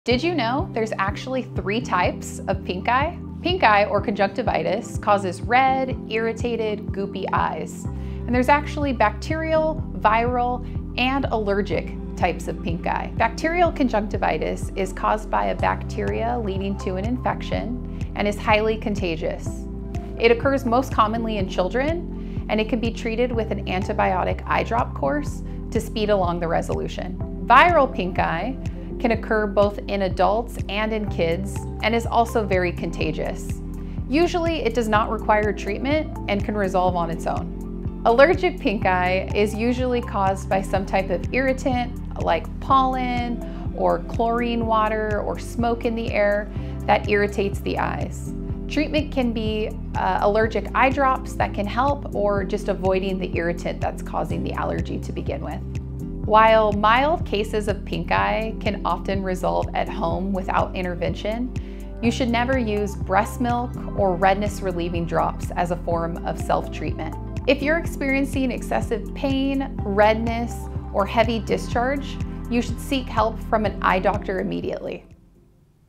3 Types of Pink Eye
Did you know there are 3 different types of pink eye?👁️ Pink eye causes red, itchy, and goopy eyes when affected. There is bacterial, allergic, and viral pink eye or conjunctivitis.

🦠 Bacterial conjunctivitis is caused by certain bacteria and leads to eye infections. This type of pink eye is the most common in children and schools, due to how contagious it is. This type of pink eye can be treated with antibiotic eye drops.

🧬 Viral conjunctivitis is caused by a virus and is common in both children and adults. Usually, it resolves on its own without any treatment.

🤧 Allergic conjunctivitis is caused by an irritant that causes an allergy (pollen, chlorine water, etc.) and is also common with adults and children. Allergic eye drops and avoiding the irritant can help this type of pink eye.

Most cases of mild pink eye can resolve on their own, however, there are two things you should not use to treat your pink eye. 🚫 🍼 Never use breastmilk or redness-relieving drops. 🚫 💦

If you are also experiencing excessive pain, redness, or discharge with your pink eye; contact an eye doctor immediately.